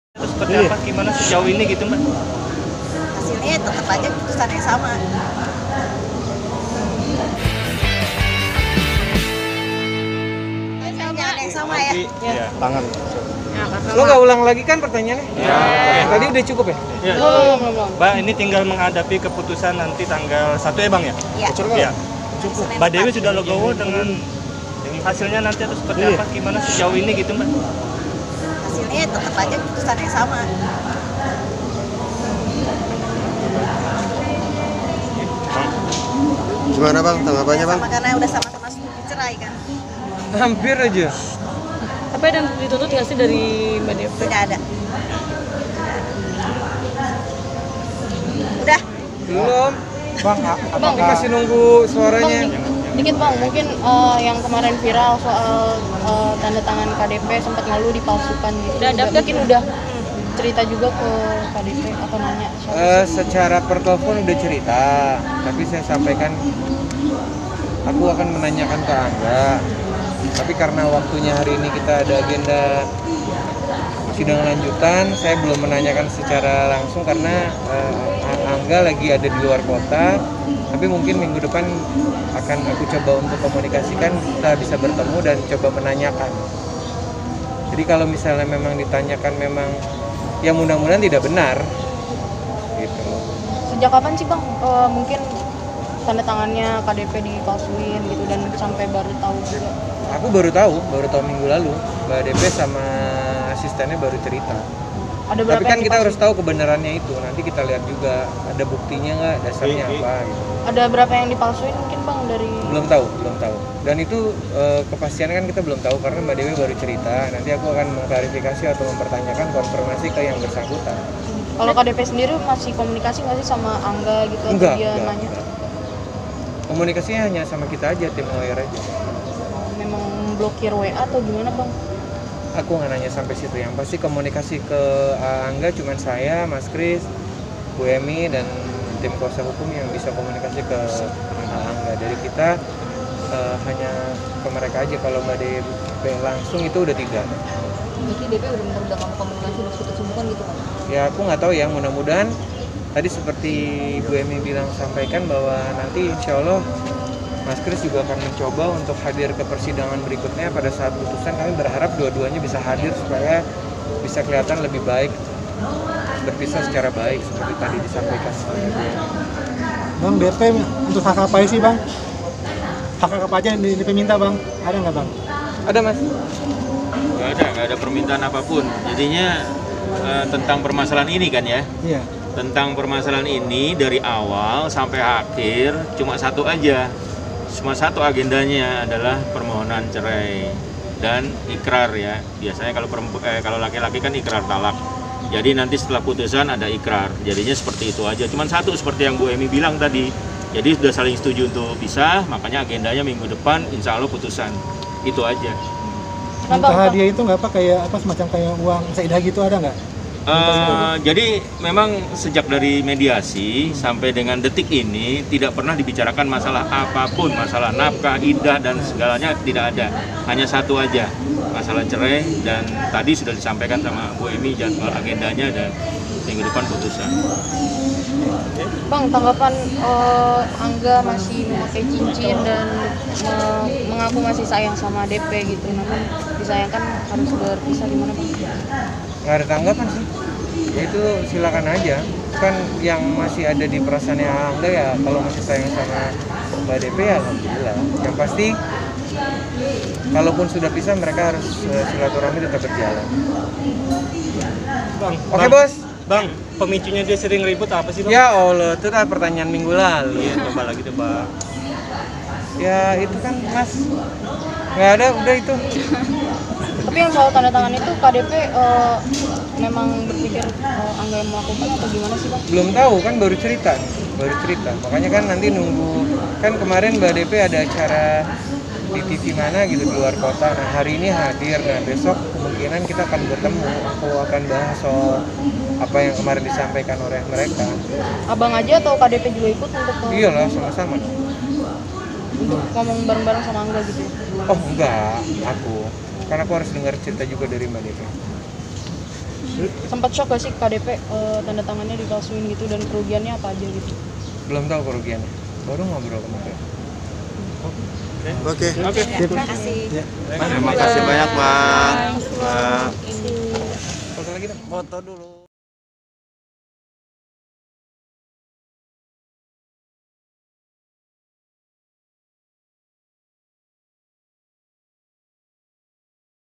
DEWI PERSIK TETAP KUKUH INGIN BERPISAH DARI ANGGA WIJAYA
Dewi persik tetap kukuh ingin berpisah dengan angga wijaya